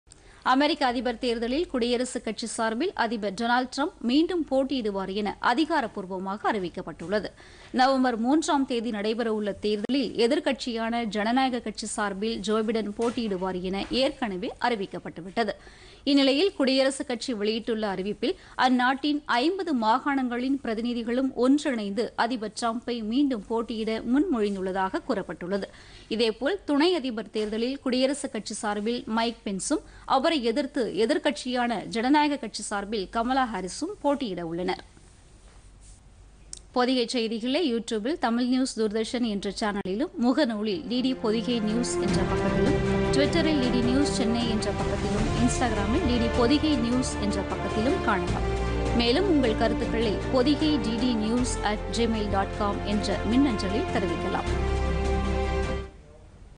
குடியரசுக்கட்சி அதிகாரப்பூர்வ அறிவிப்பு
அமெரிக்க அதிபர் ‍தேர்தலில்  மீண்டும் போட்டியிடுகிறார் டொனால்டு டிரம்ப் :  குடியரசுக்கட்சி அதிகாரப்பூர்வ அறிவிப்பு